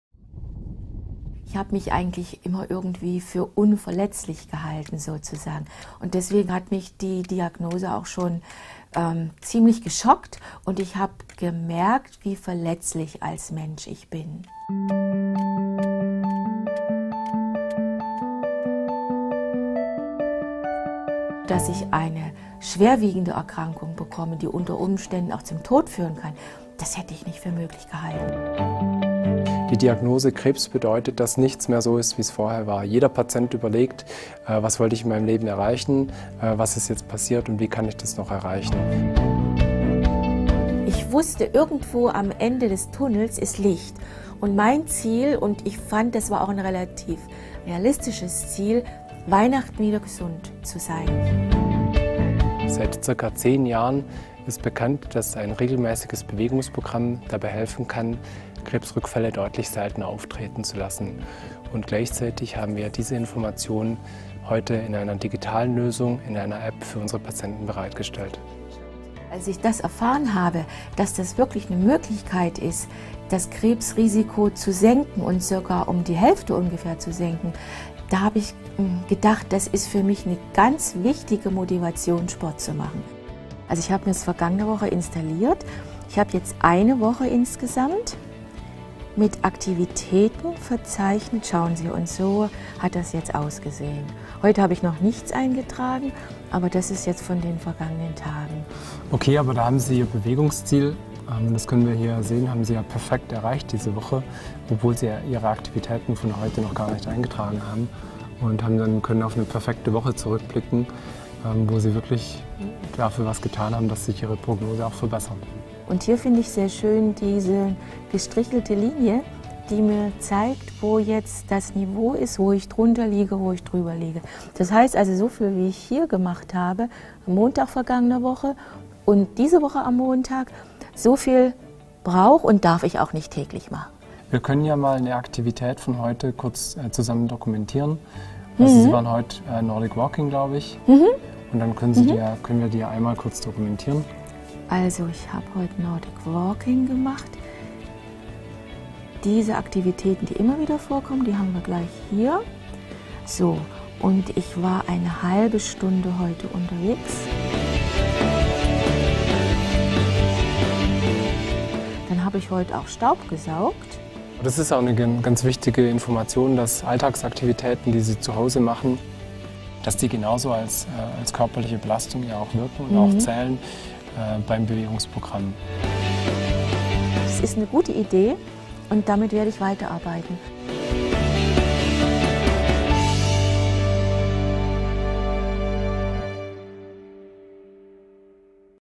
Bewegung gegen Krebs/movival App | Asklepios
Diagnose Krebs! Nach ihrer Chemobehandlung entscheidet sich Ruth Michelbach für einen Reha Aufenthalt in der Asklepios Klinik Triberg. Privatdozent Dr. Thomas Widmann behandelt die Patientin mit Lymphdrüsenkrebs. Entscheidend ist für sie der Therapieansatz “Bewegung gegen Krebs”. Mit der App movival lernt Ruth Michelbach wie viel Bewegung gut für sie ist und wie sie ihr Rückfallrisiko minimieren kann.